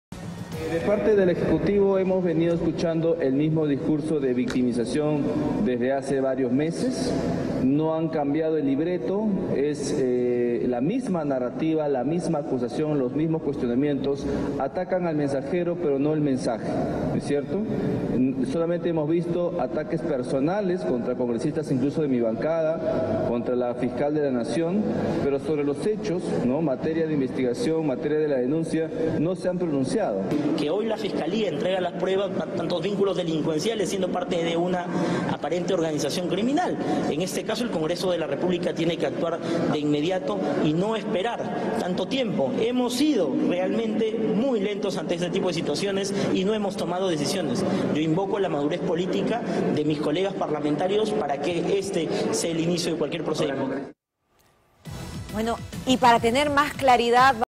Congresistas Alejandro Muñante y Diego Bazán critican actitud del Ejecutivo
Durante la mañana de hoy miércoles (12.10.22) los congresistas Alejandro Muñante y Diego Bazán, se pronunciaron respecto a la denuncia constitucional de la fiscal Patricia Benavides contra pedro castillo, el cual, ambos congresistas se mostraron en favor de la decisión por parte de la máxima autoridad del Ministerio Público.

"De parte del Ejecutivo hemos venido escuchan el mismo discurso de victimización desde hace varios meses […] atacan al mensajero, pero no al mensaje. Hemos visto ataques contra congresistas y, hasta a la fiscal de Nación, pero sobra la denuncia y los hechos no se han pronunciado”, expresó el legislador Alejandro Muñante.

Por su parte, Diego Bazán, legislador de Avanza País, se mostró de acuerdo con la decisión de la fiscal de la Nación, Patricia Benavides, señalando que, “el congreso tiene que actuar de inmediato y no esperar que pase más tiempo”. “Invoco a mis colegas parlamentarios para que este (denuncia constitucional) sea el inicio de cualquier procedimiento”, señaló.

DENUNCIA CONTRA PEDRO CASTILLO

La Fiscal de la Nación, Patricia Benavides, presentó ayer martes (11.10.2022) una denuncia constitucional en el Congreso contra el presidente Pedro Castillo por liderar una supuesta organización criminal, así como por tráfico de influencias y complicidad en un presunto caso de fraude. El documento de la Fiscal deberá ahora ser evaluado por la Subcomisión de Acusaciones Constitucionales del Parlamento que, de determinar su procedencia, pasará a la Comisión Permanente hasta llegar al pleno del Congreso.